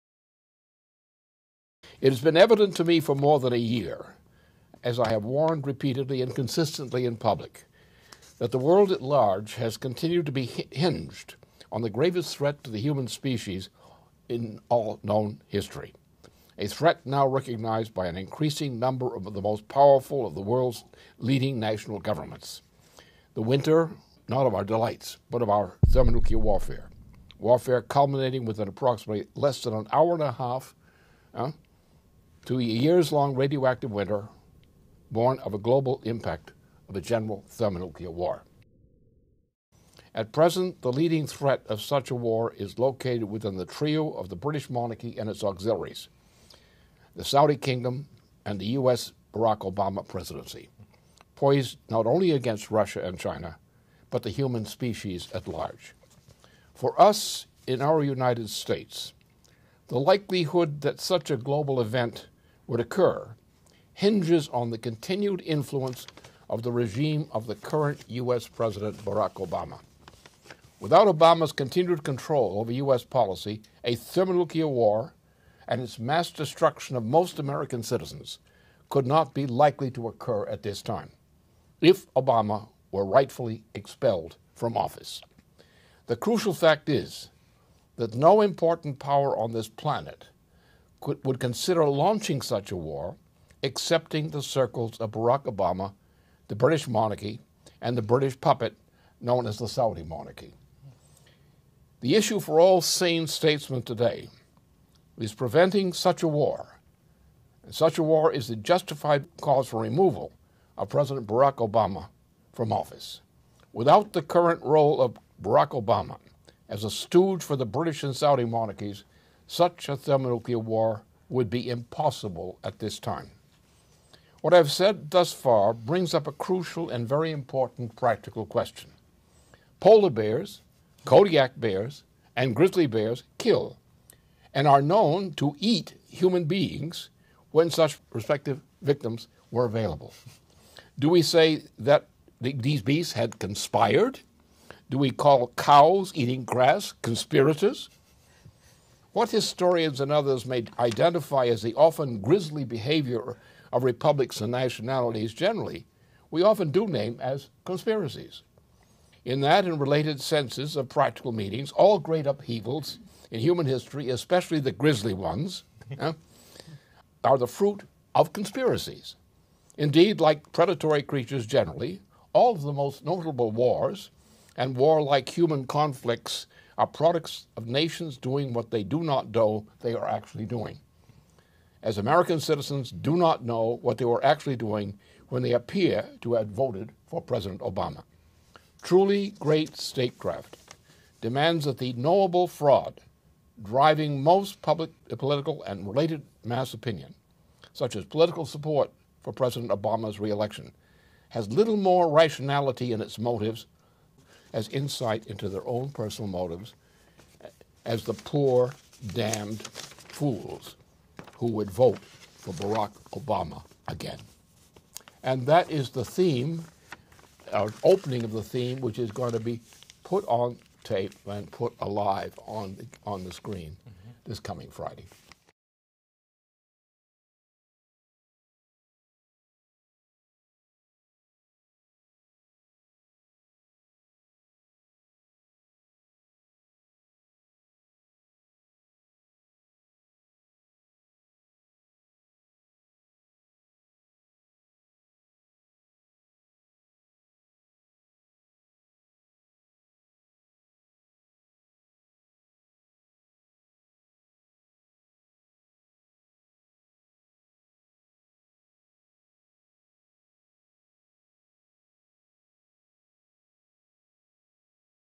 Trailer • Videoconferencia internacional de Lyndon LaRouche
Trailer de la próxima videoconferencia de Lyndon LaRouche en vivo, este próximo viernes 5 de octubre a las 21 horas de Argentina y 19 pm de México, con traducción simultánea en LaRouchista.com